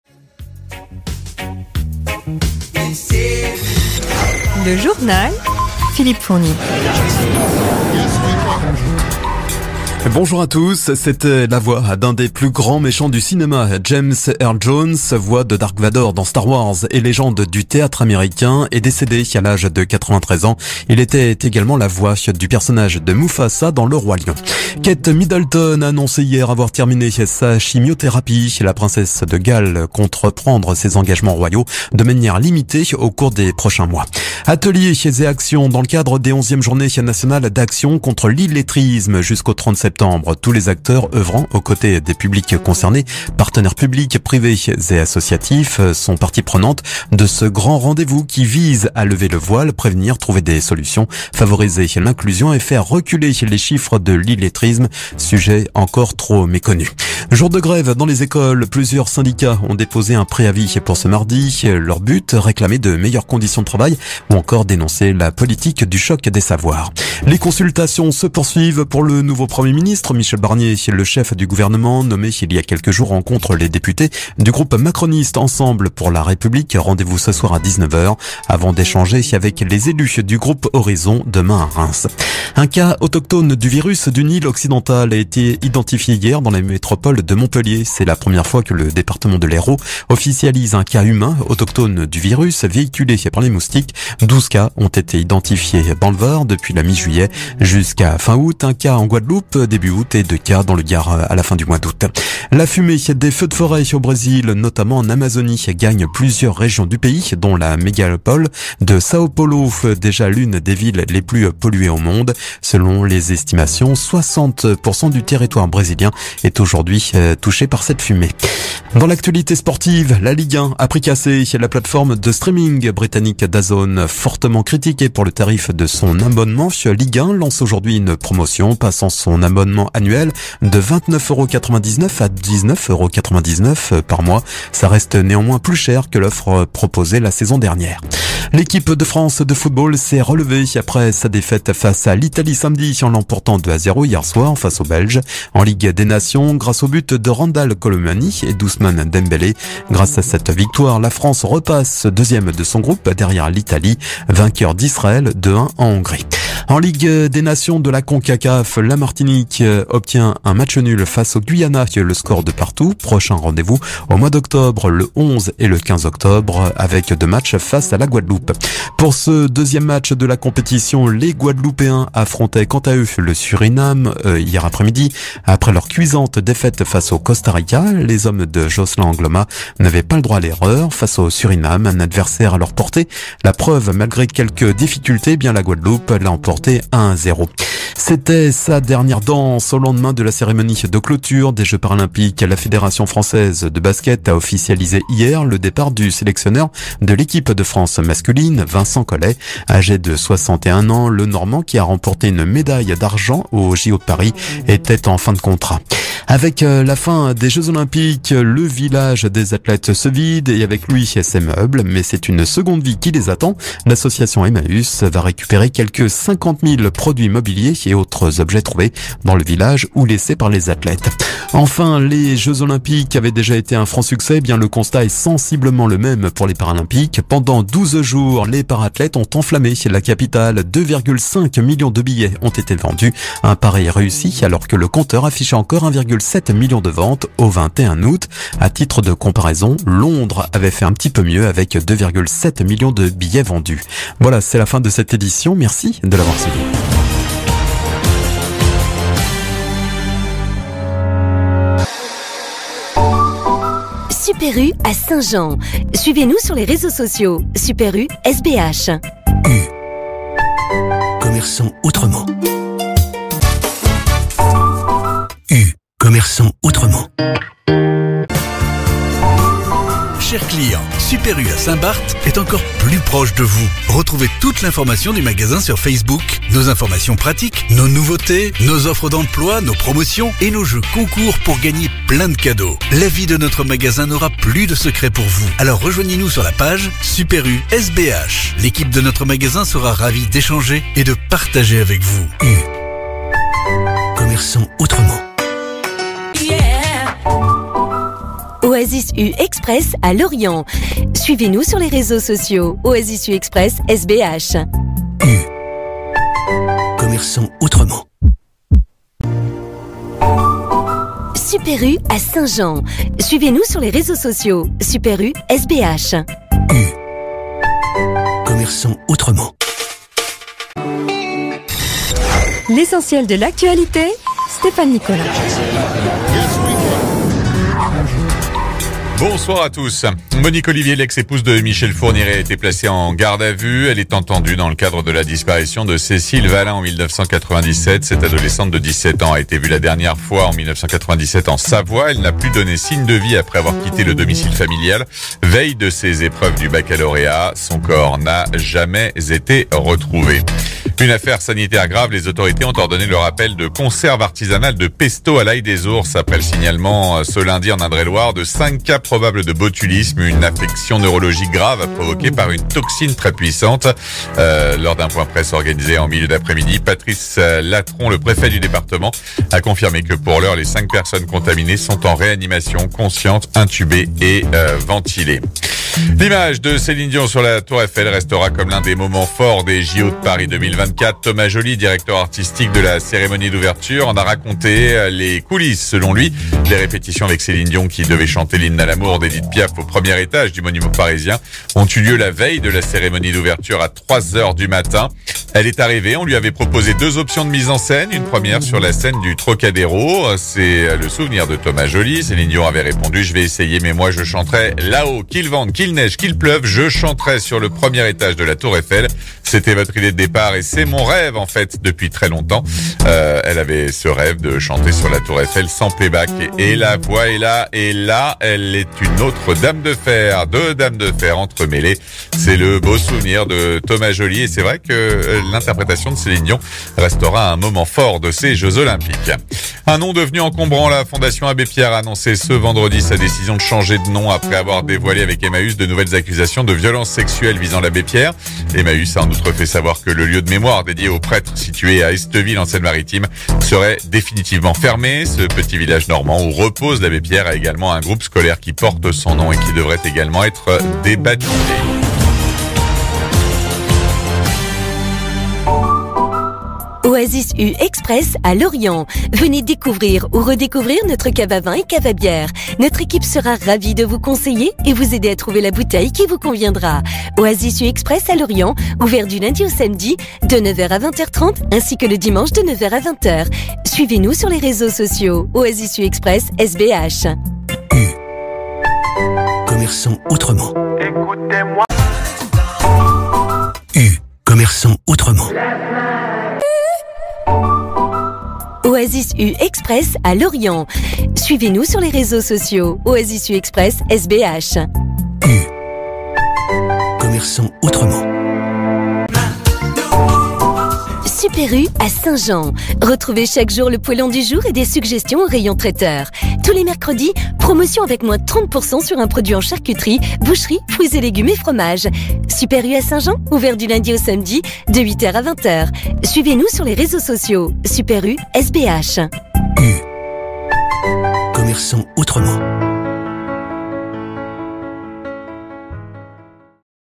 Radio U Saint Barth. (Guadeloupe) - extrait radio interne
Extrait de Radio U du 10/09/2024 entre 12h02 et 13h30 (heure de la Guadeloupe).

Radio U est une radio interne qui est diffusée dans les 2 supermarchés U de l'île de Saint Barthélémy, en Guadeloupe.

L'écoute de cette radio est en stéréo et ouverte 24h/24.

La radio était en direct pendant l'enregistrement.

Nombre d'abonnés à la naissance de cette vidéo : 809